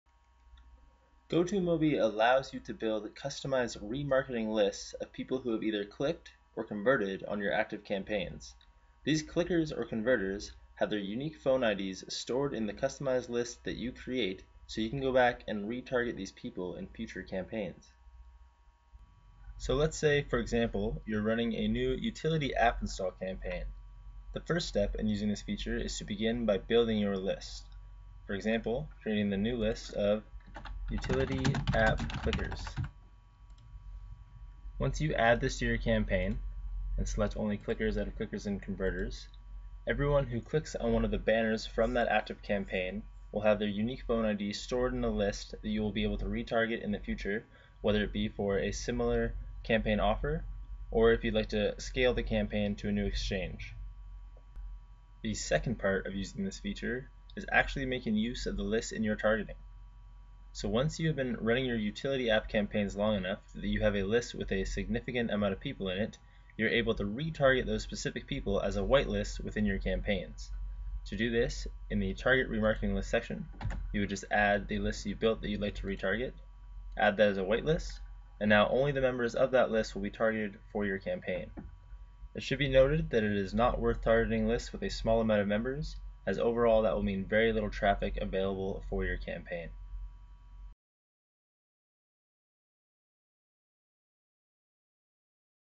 Go2mobi Academy - Remarketing Lists
Watch this short video to learn about how to use Go2mobi's Remarketing lists.